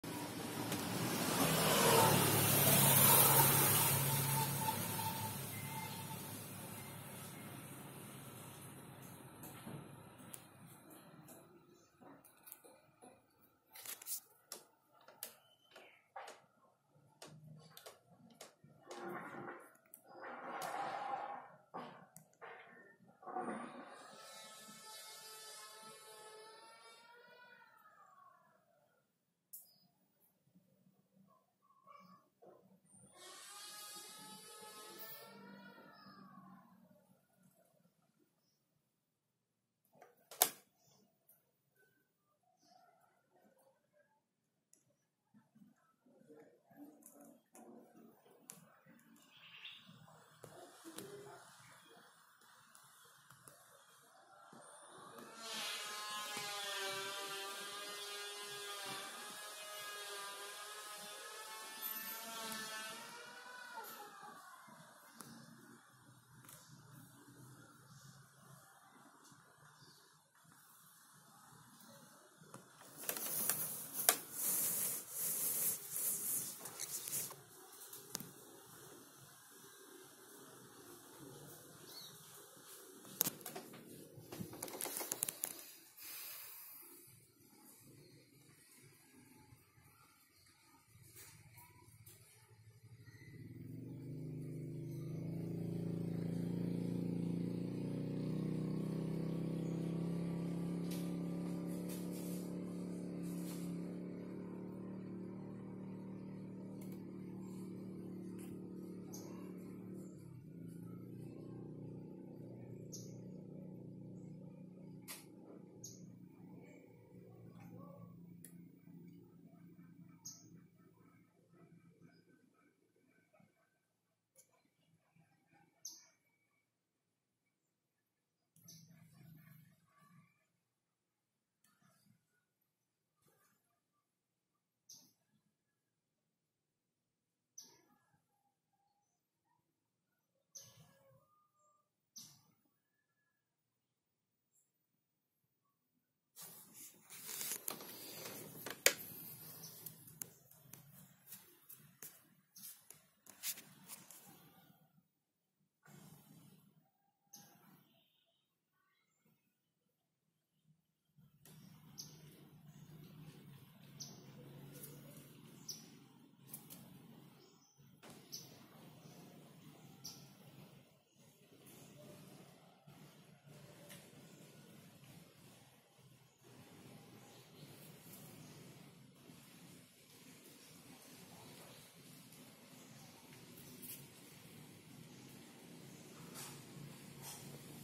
Olive Tree Skink.
Kadal pohon hijau adalah sejenis kadal yang hidup di pohon. Kadal ini tersebar luas di kepulauan Nusantara. Nama kadal ini dalam bahasa Inggris yaitu Olive Dasia atau Olive Tree Skink.

Nama ilmiah: Dasia olivacea

Ordo: Squamata

Famili: Scincidae

Spesies: D. olivacea